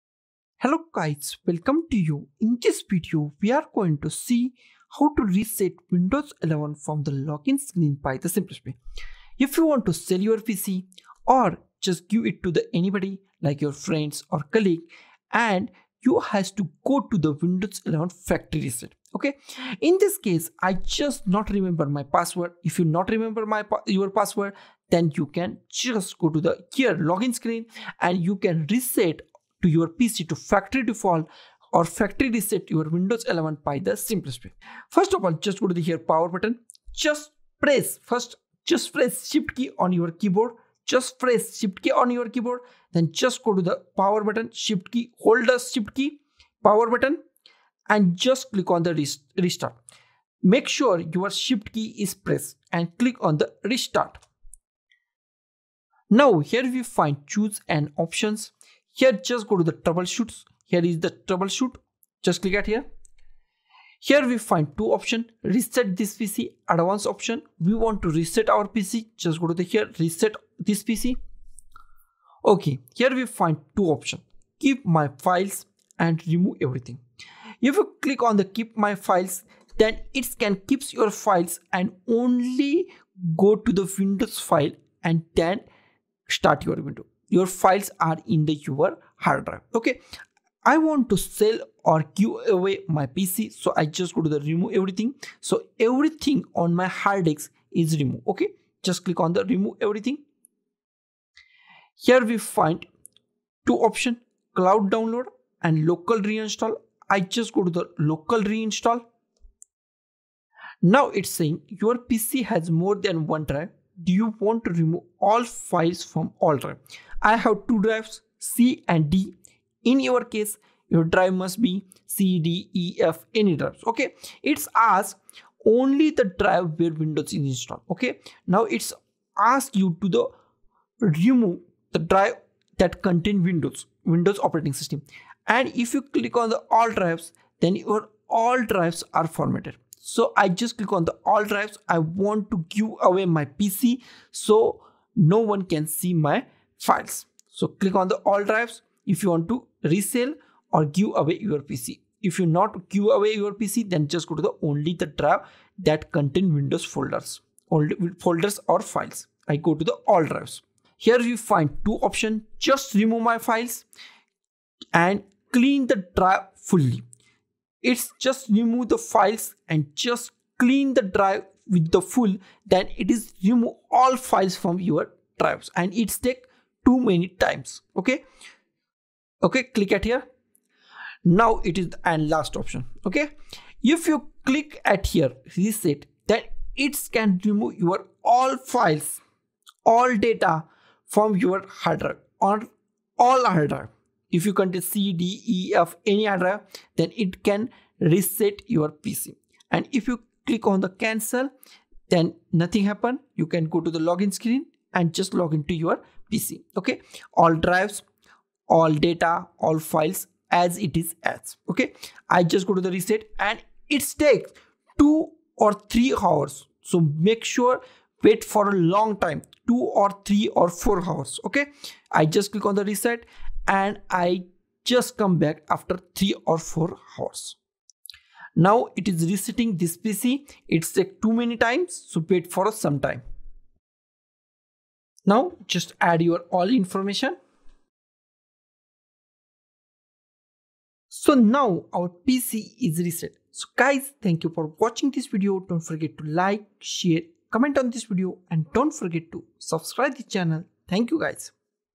How To Reset Windows 11 From The Login Screen
In this video We are going to see How to Reset Windows 11  PC or Laptop without Logging in. So let us see  How to Reset Windows to Factory Settings without installation disc. Its easy and straight forward  to factory reset Windows 11 without knowing the password.

This instructions in this video tutorial can be applied  for  laptops, desktops, computers, and tablets which run the Windows 11 operating system like Windows 11 Professional,  Windows 11 Enterprise, Windows 11 Home, Windows 11 Education. This tutorial will work on all hardware manufactures, like Dell, Asus,  Acer,  HP, Toshiba, Lenovo, and Samsung.